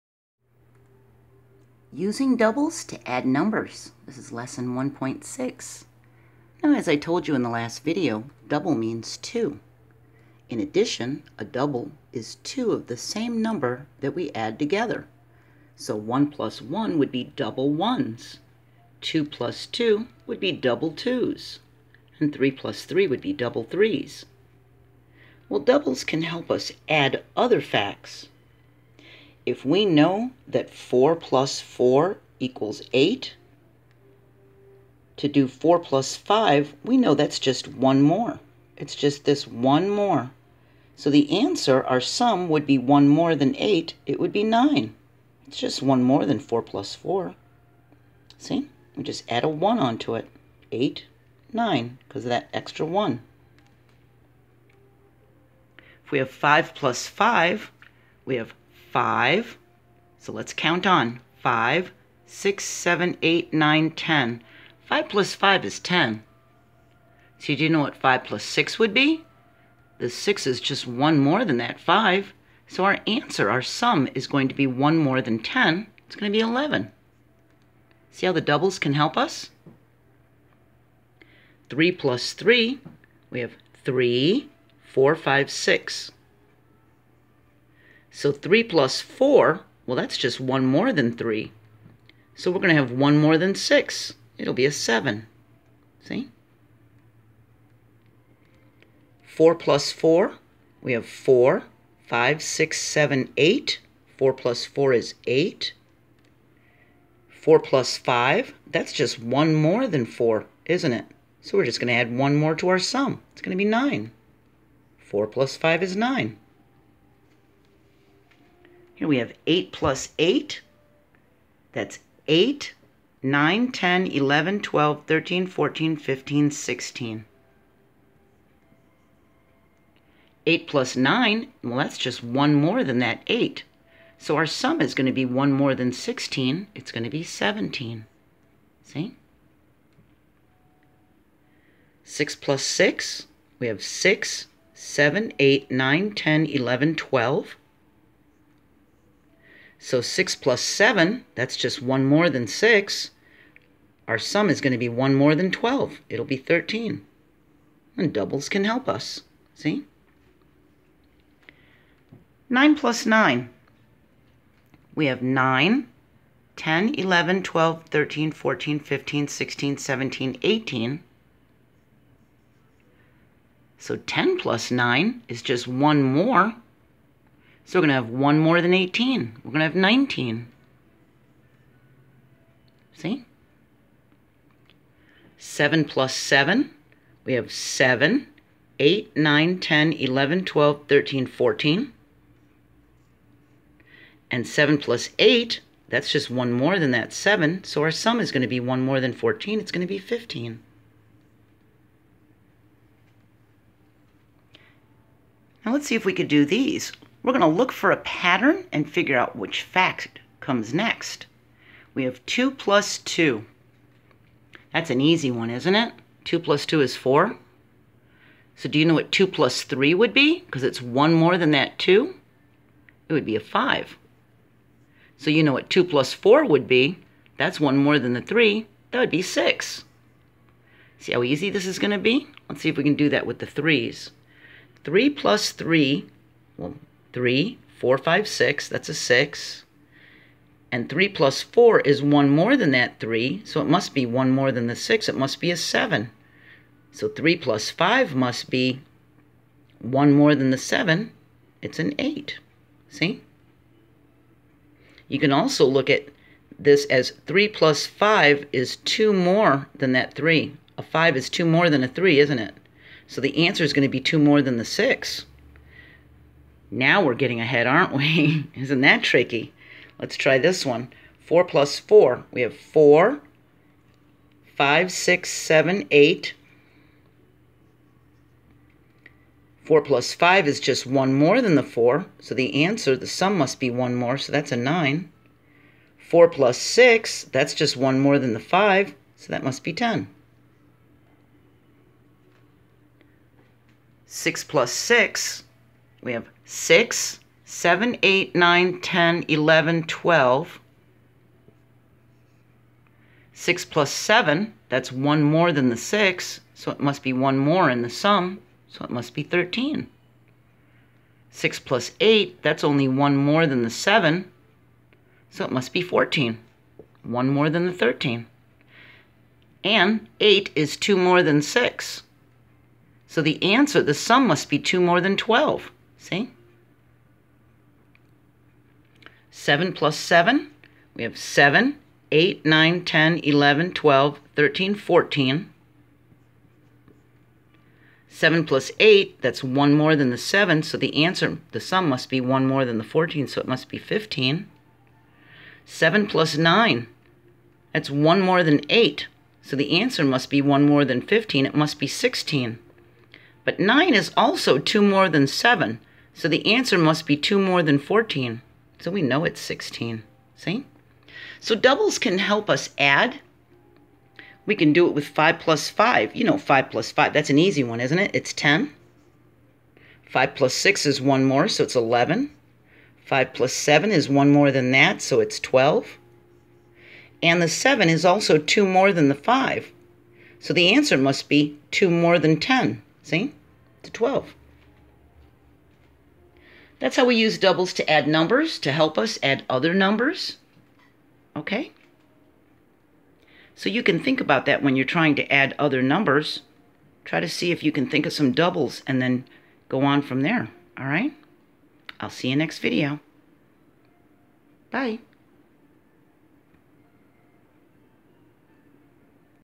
Grade 2 Math 1.6, Using Doubles to Add numbers
How to add double numbers and how doubles can help us add other amounts. Page 11 in the Silver Burdett Grade 2 textbook.